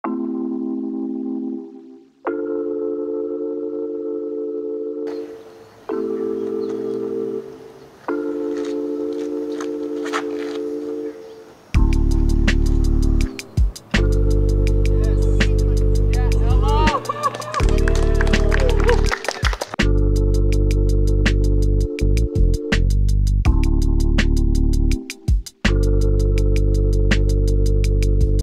Ella Hansen Hole 17 | USWDGC | Rocklin, California
During the final round of the United States Women's Disc Golf Championships, Ella Hansen threaded the needle and put it about as close as possible on Hole 17. Hole 17 is a 309 ft. Par 3 that has a real tight gap late.

Follow Ella on Instagram @onethrowella and on Twitter @BagelPowered